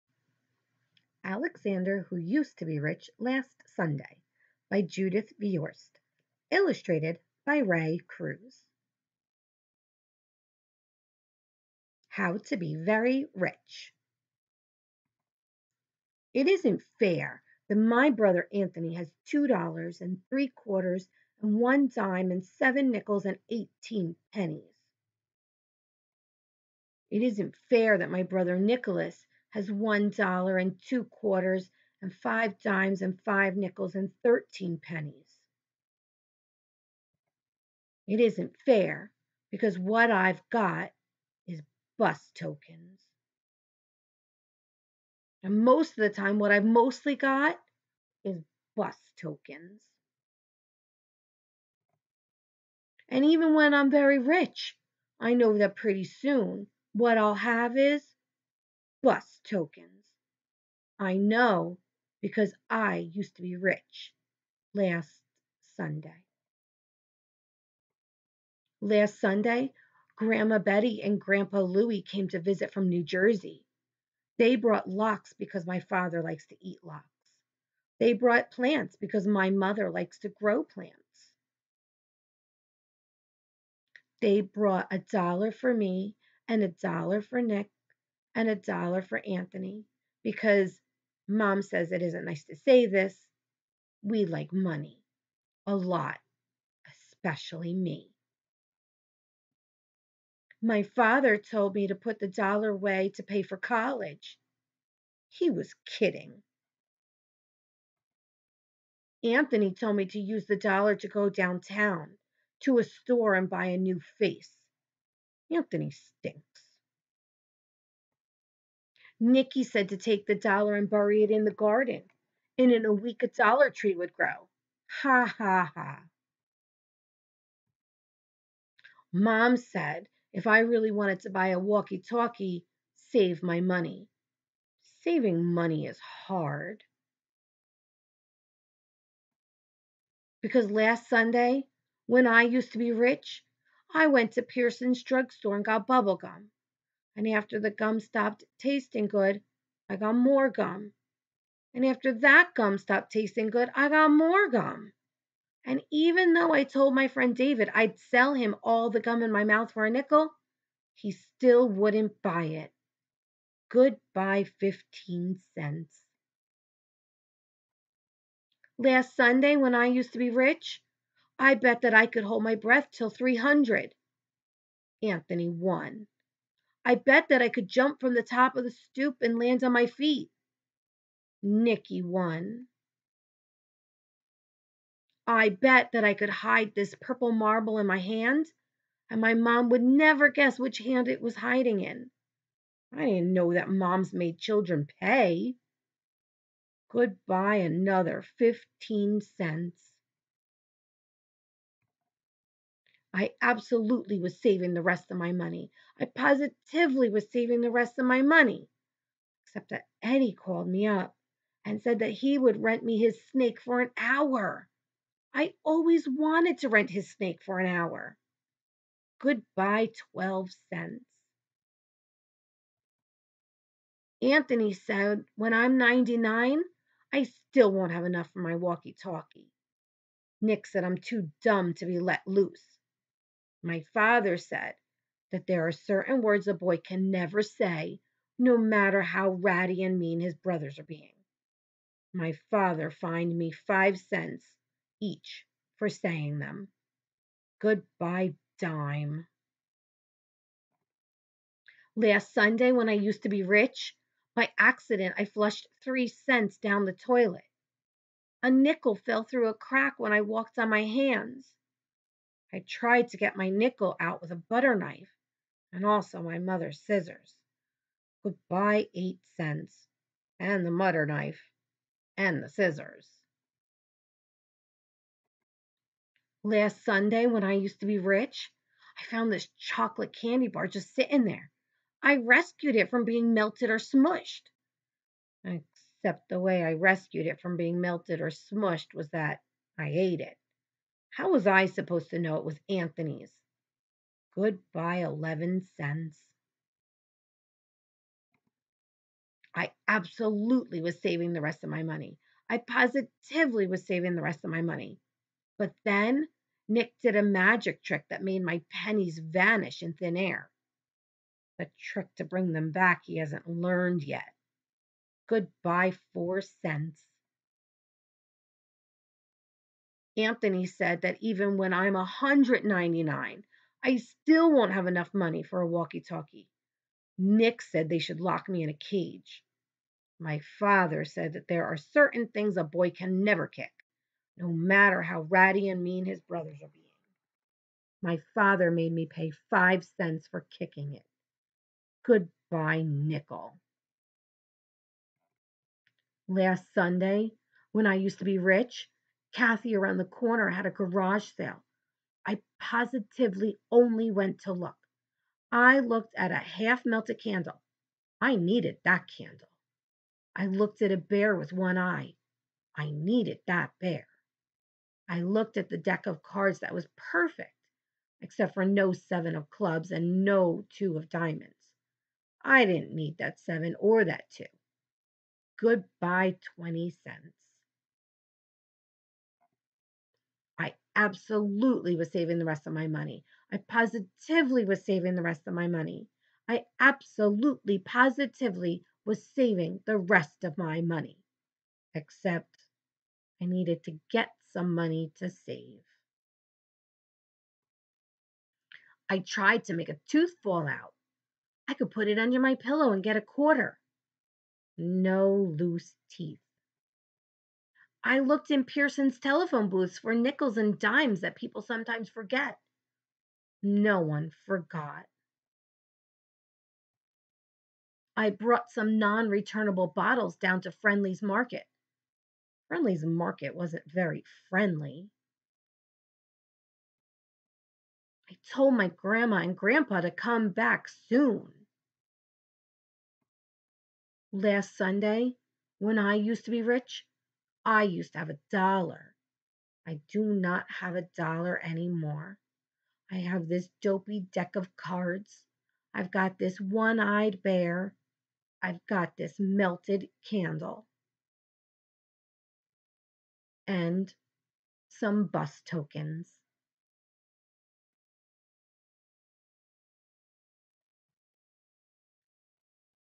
Alexander Who Used to be Rich Last Sunday
Alexander just doesn't know how to save his money.